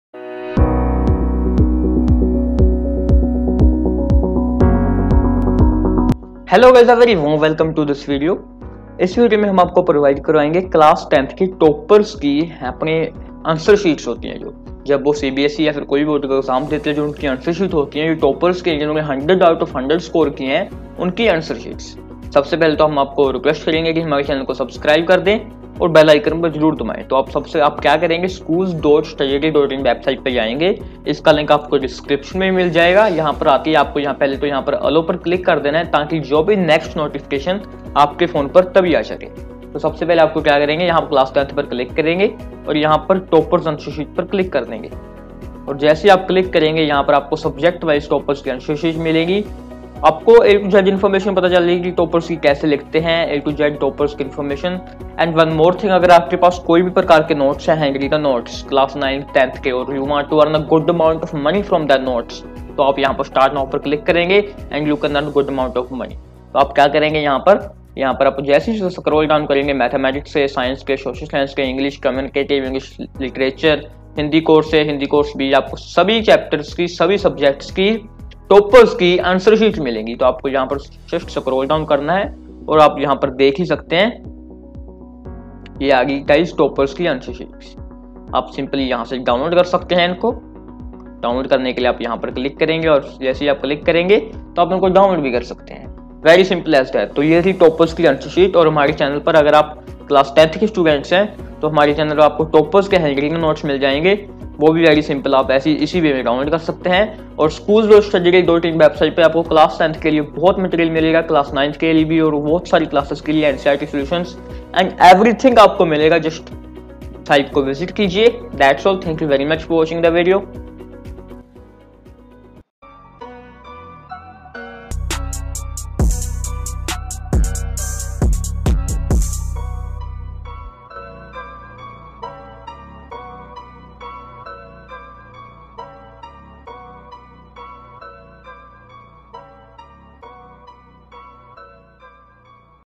Class 10: Topper's Answer Sheet Download PDF for FREE - Class 10 Topper's Sheets
Download CBSE Class 10 Topper's Answer Sheet Download PDF for FREE - Class 10 CBSE Topper Answer Sheet 2020-21. Class 10 Topper Answer Sheet of Science, Maths, SST, English, and Hindi All Subjects.